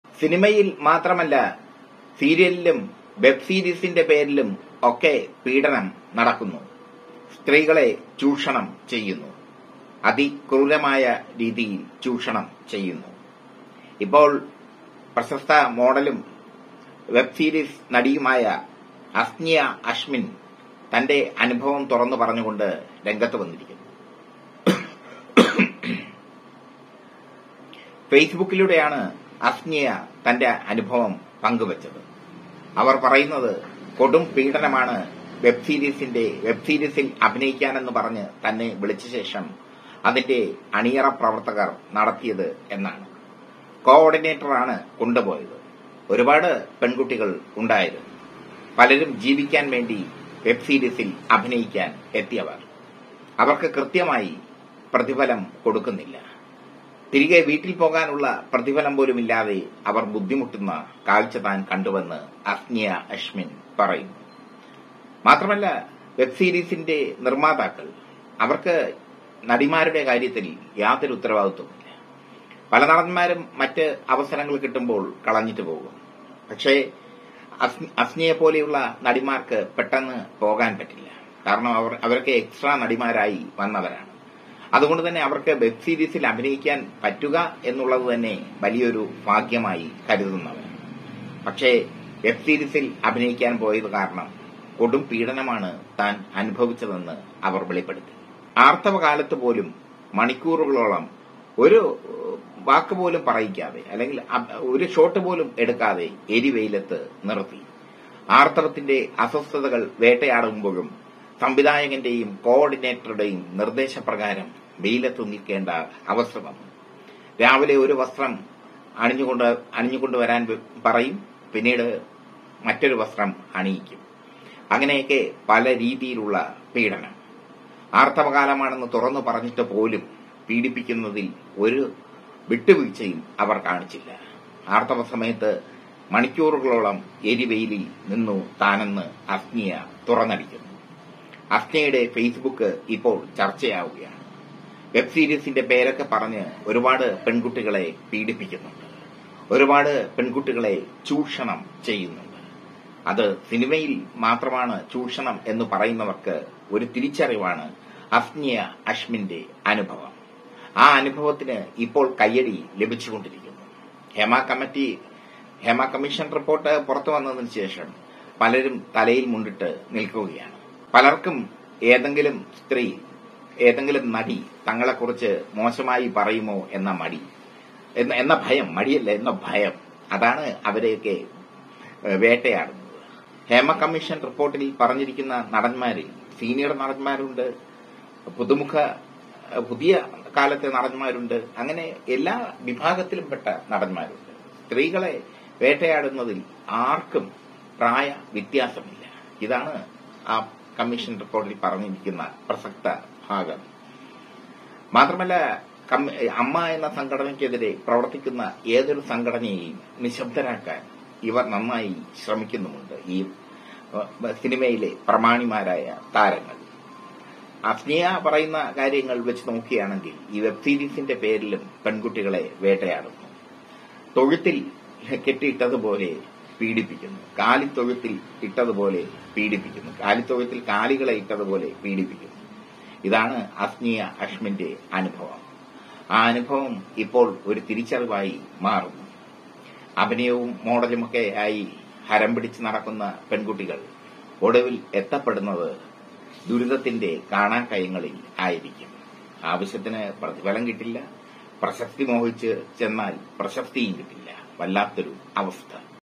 ആർത്തവകാലത്തും ചൂഷണം | നടി അസ്നിയ അഷ്മിന്റെ അനുഭവം
ആർത്തവകാലത്തും ചൂഷണം
നടി അസ്നിയ അഷ്മിന്റെ അനുഭവം